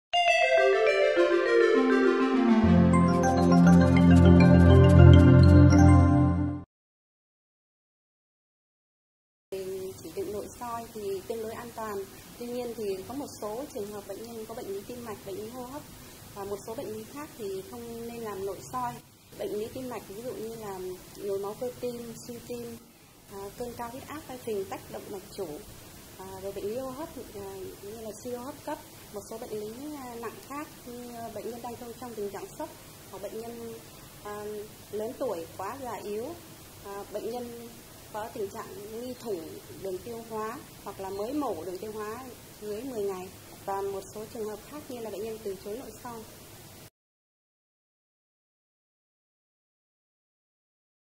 Những ai không nên nội soi tiêu hóa
Nội soi tiêu hóa không có chống chỉ định tuyệt đối. Tuy nhiên bác sĩ có thể hoãn nội soi khi nghi ngờ: thủng đường tiêu hoa, bỏng do uống acid, suy tim, thiếu máu cơ tim cấp, suy hô hấp hoặc mới ăn no; chất barium còn sót lại trong ống tiêu hóa sau khi chụp dạ dày cản quang sẽ gây khó khăn cho nội soi đường tiêu hóa trên; những bệnh nhân có túi phình lớn ở động mạch chủ, túi thừa Zenker (là một túi thoát vị ở thực quản), loét thủng mới xảy ra gần đây, hoặc thủng ở những nơi khác trong ống tiêu hóa...

Hãy gửi những thắc mắc của bạn về cho chúng tôi, bạn sẽ được các bác sĩ đúng chuyên khoa tư vấn, giải đáp hoàn toàn MIỄN PHÍ.